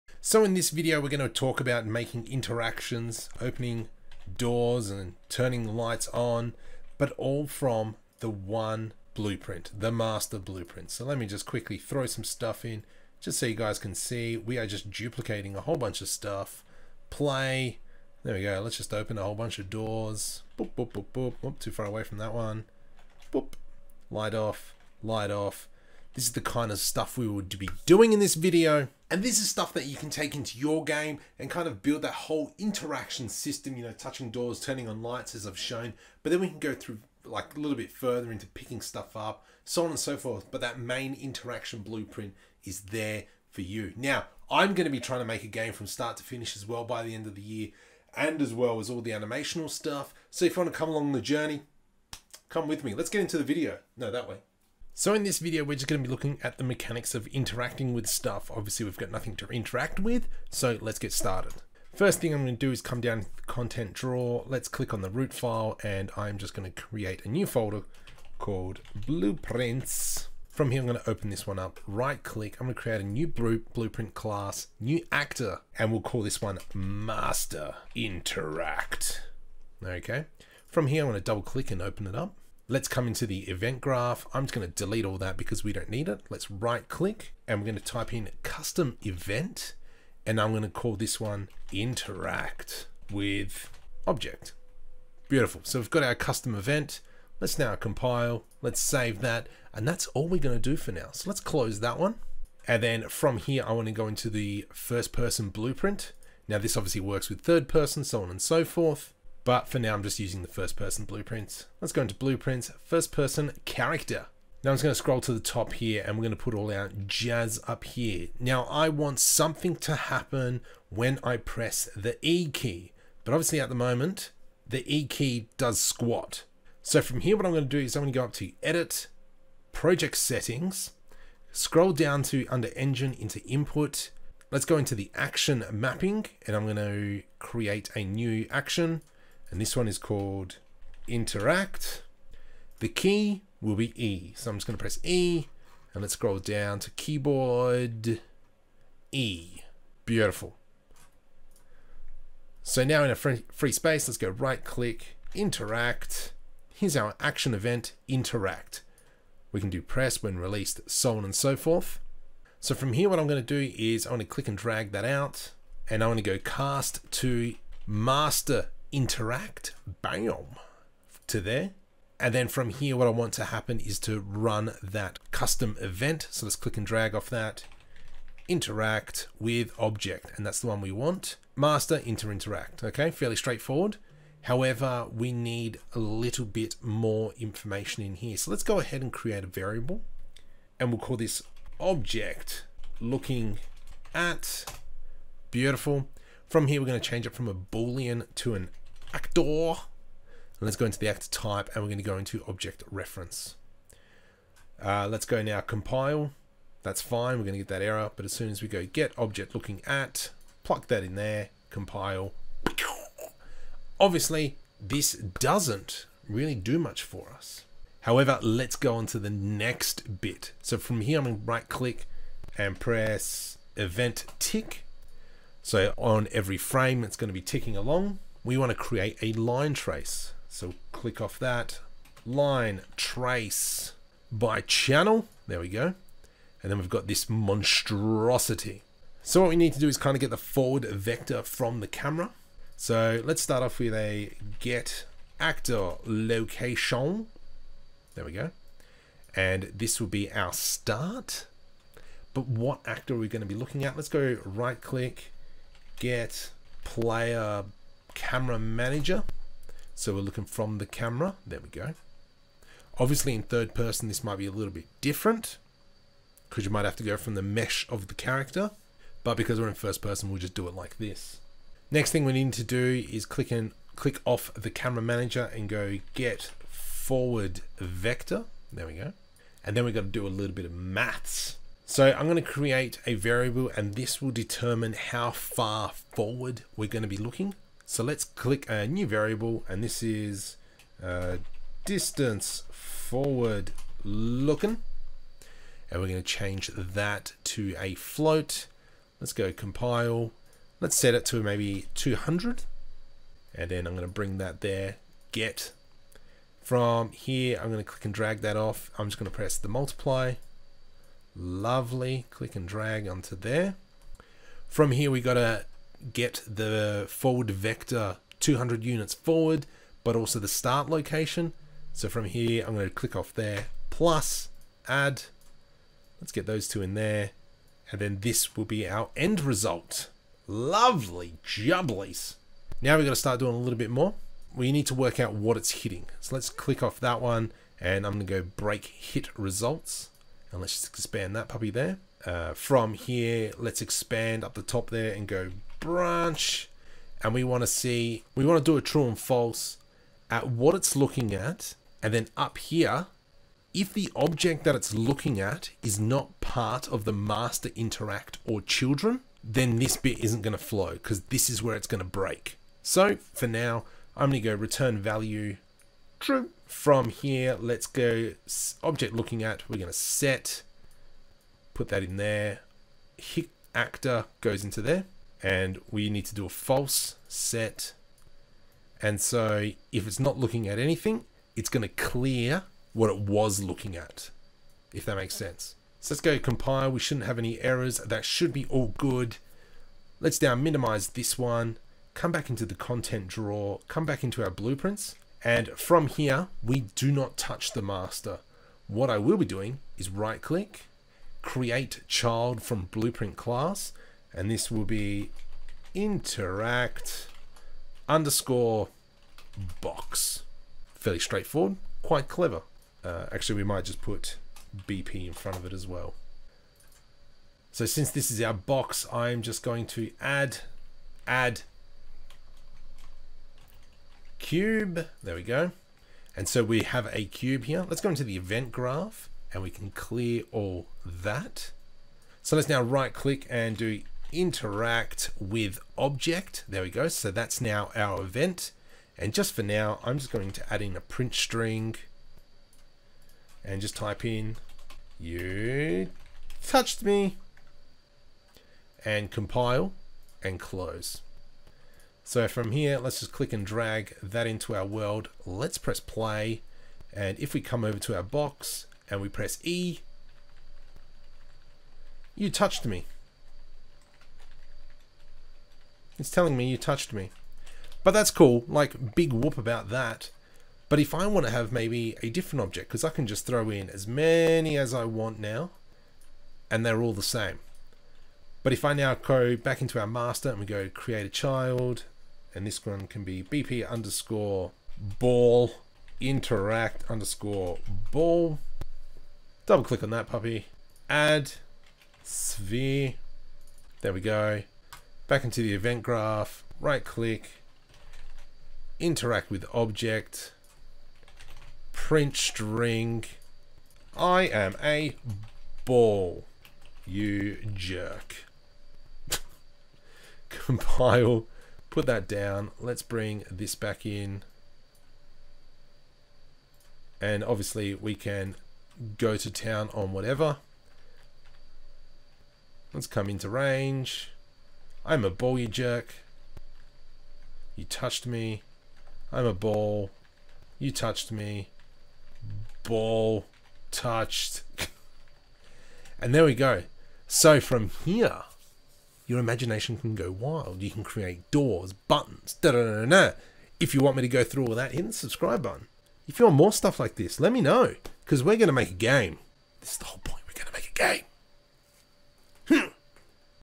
Setting up Interaction with Blueprints Unreal Engine 5 Tutorial
Discord Server Below

In this video we will set up blueprints in Unreal Engine 5 and 4 on how to interact with objects in a game that will span across children so that no manually setup is required. This will also sets up communication between the player and other blueprints/actors

Also, yes, my computer was screaming out in pain when I was doing this.